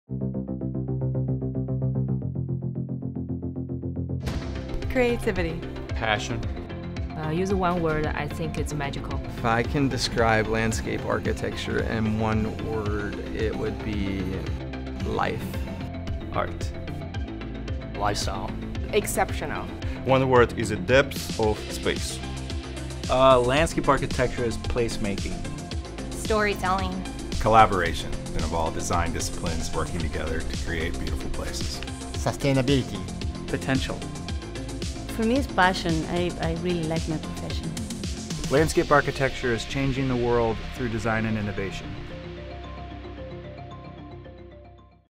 EDSA Landscape Architecture is...
In honor of World Landscape Architecture Month 2018, we asked EDSA employees what landscape architecture means to them.